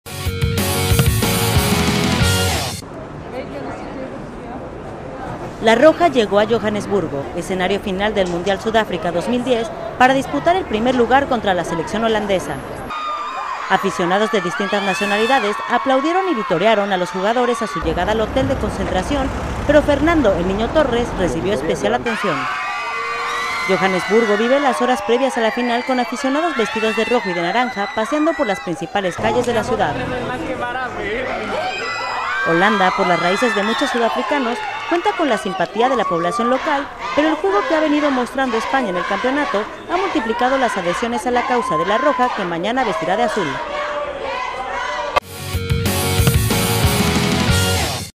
SUDÁFRICA 2010 La selección española llega a Johannesburgo,
Johannesburgo, 10 jul (EFE) (Imágenes: Ramón Martínez).- Un centenar de aficionados se dieron cita hoy a las puertas del hotel Da Vinci, en el barrio financiero de Sandton, para contemplar la llegada de España a Johannesburgo, escenario de la final.